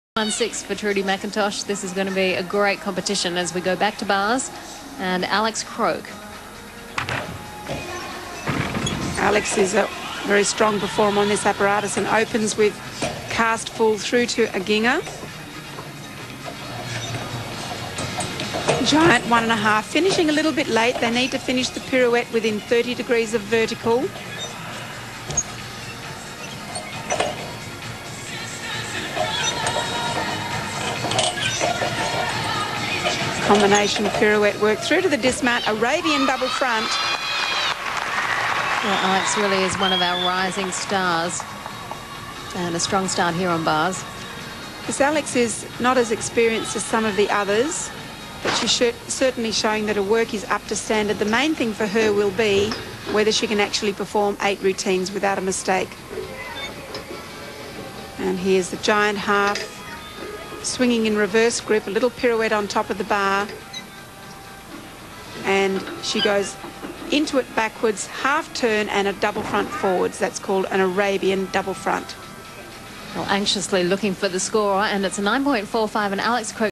Alex Croak 2000 Olympic Trials Day 1 Bars
Bar routine of Alex Croak from NSW on Day 1 of the 2000 Australian Olympic Trials.